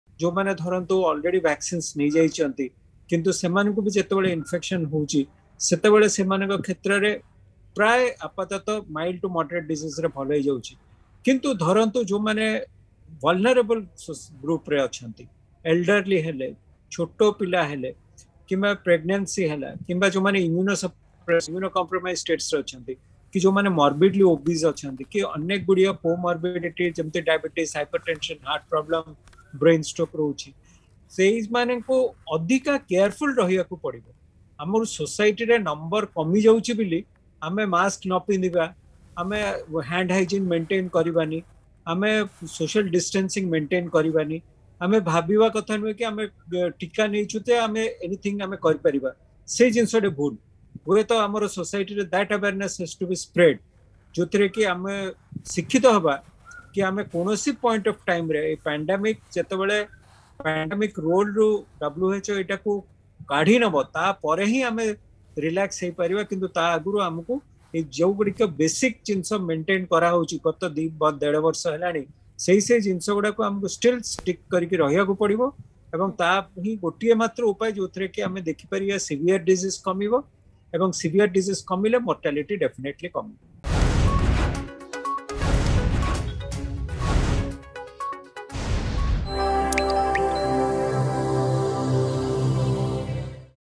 ସଂକ୍ରମଣ କମିଲା ପରେ ମଧ୍ୟ ସଚେତନ ରୁହନ୍ତୁ
ସଂକ୍ରମଣ କମିଲା ପରେ ଗର୍ଭବତୀ,ଛୋଟ ପିଲା ଓ ଅନ୍ୟ ରୋଗରେ ପିଡିତ ଥିବା ବ୍ୟକ୍ତି ରୁହନ୍ତୁ ସଚେତନ